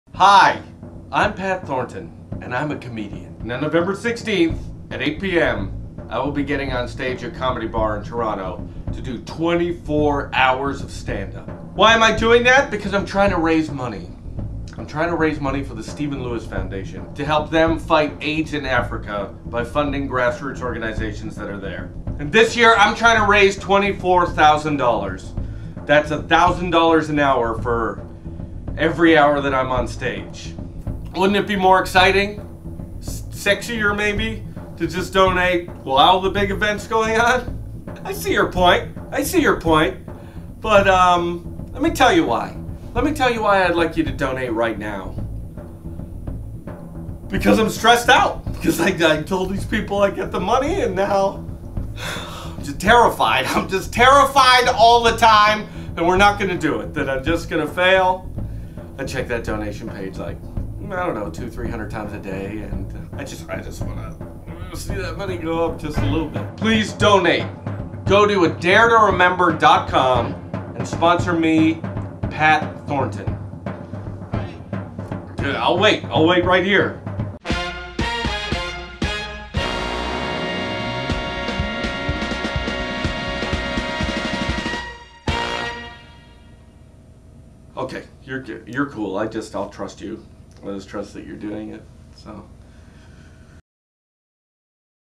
Pat Pleads
Comedian Pat Thornton tells you why to donate early to his 24 hours set for AIDS relief in Africa.